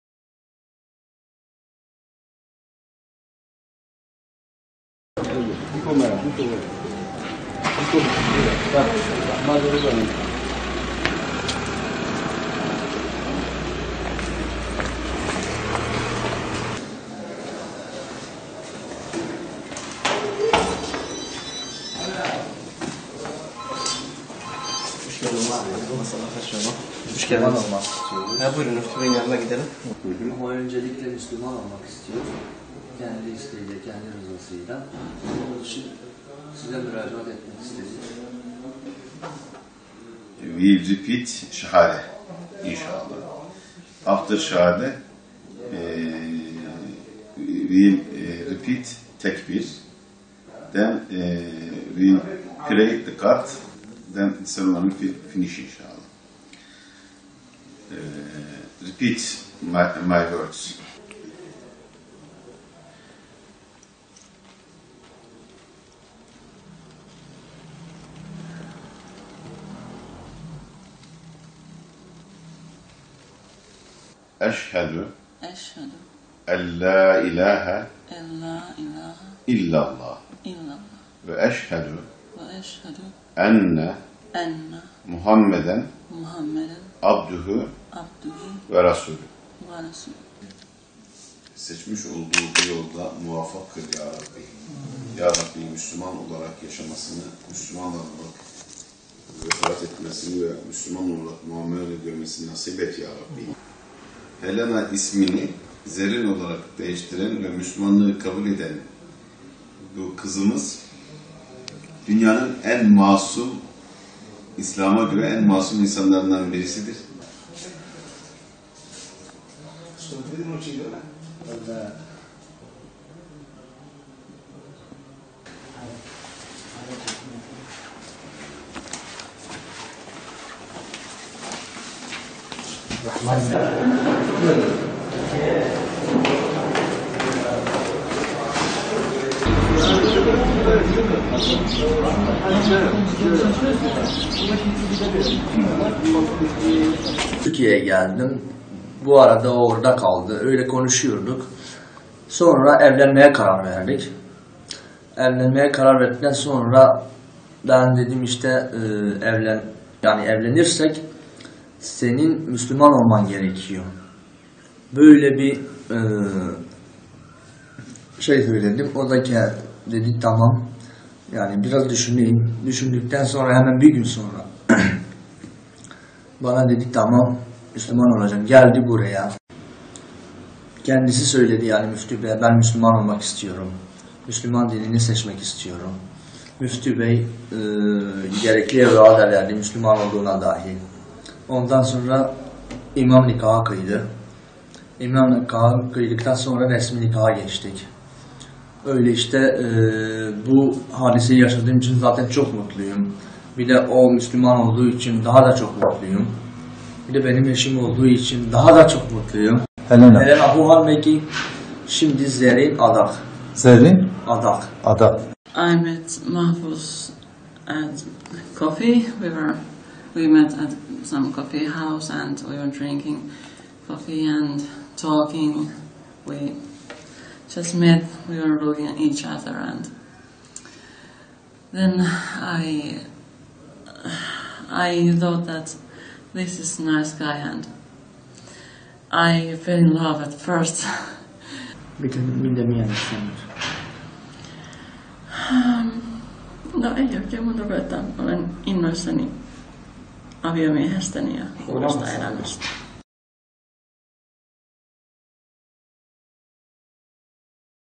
Finlandiyalı Genç Kız, Sevdiği Erkek İçin Müslüman Oldu
Finlandiyalı Hristiyan genç kız Huuhanmaki, aşık olduğu Cizreli Adak ile evlenmek için binlerce kilometre yol katedip Müslüman oldu.